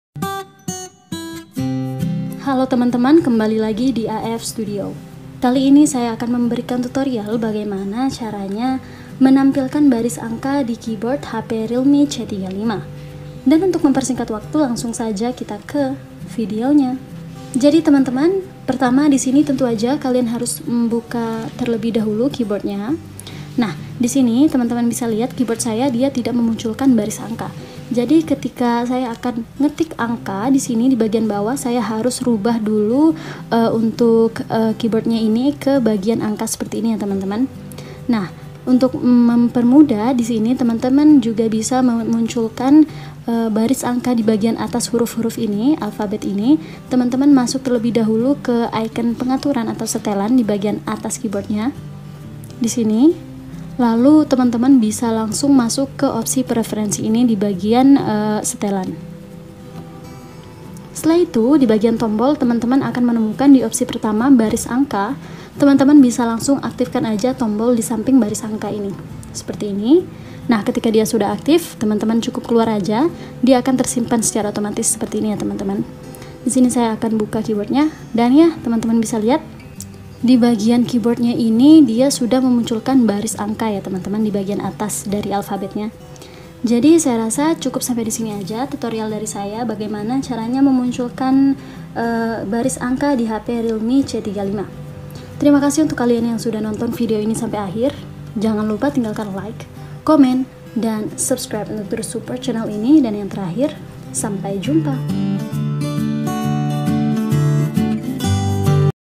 Cara Menampilkan Baris Angka Di Keyboard Hp Realme C35
Berikut saya bagikan tutorial cara menampilkan baris angka di keyboard hp realme c35. Tutorial ini bertujuan untuk memunculkan angka di atas keyboard hp realme.